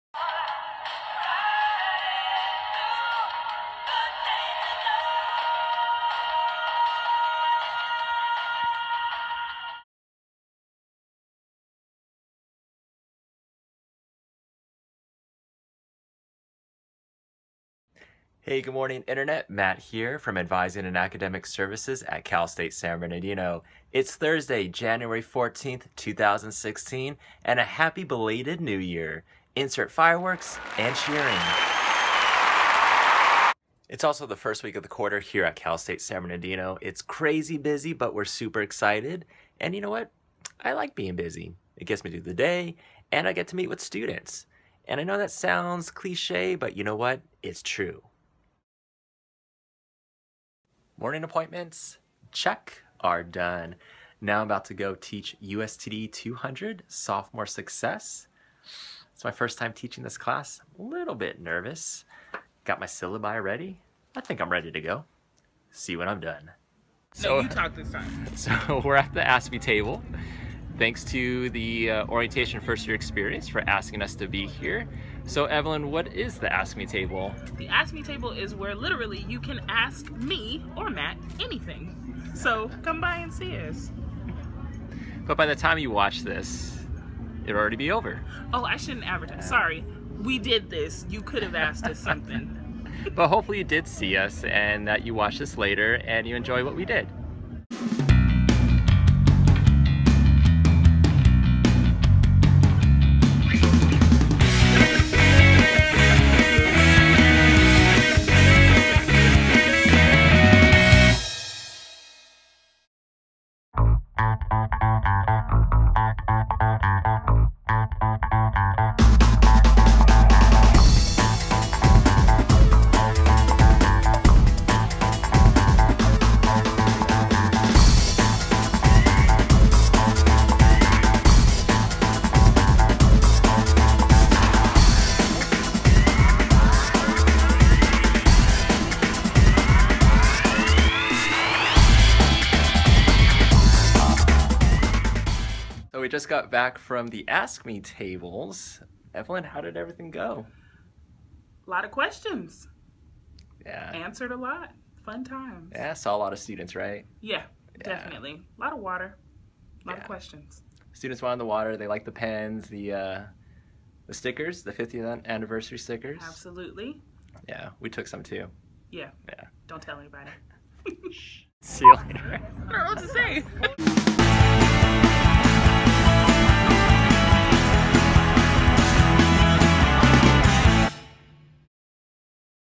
Matty's CSUSB Vlog: A day with Matt & Evelyn
Happy New Year! Vlog shot on Thursday, January 14, 2016 from academic advisors Matt Markin and Evelyn Knox!